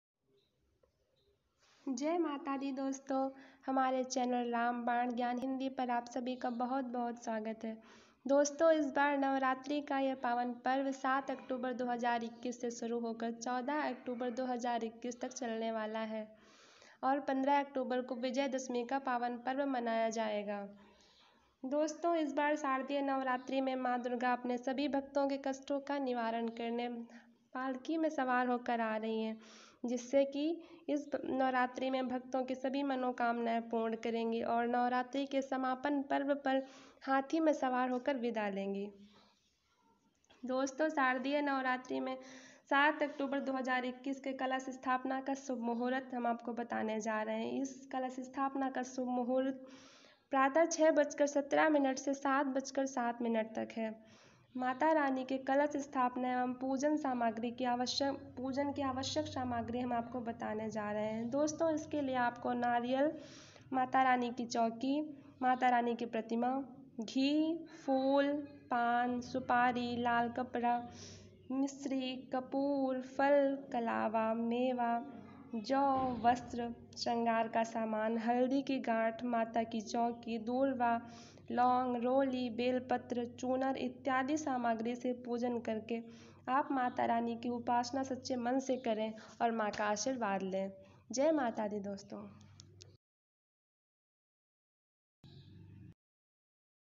नवरात्रि 2021 कलश स्थापना और ऐसे करे पूजा , पूरी होगी हर मनोकामना । नवरात्रि 2021 पूजा सामाग्री
दोस्तो मैंने इस वीडियो में नवरात्रि 2021 की कलश स्थापना के मूहूर्त और पूजा सामग्रियों के बारे में बताया है। माँ के पूजन मे क्या क्या सामाग्री लगती है वो सभी लिखी हुई है आप सभी उसी अनुसार पूजा करके अपनी मनोकामना के लिए माता की वंदना करे ।माता आपकी मनोकामना जरूर पूरी करेगी ।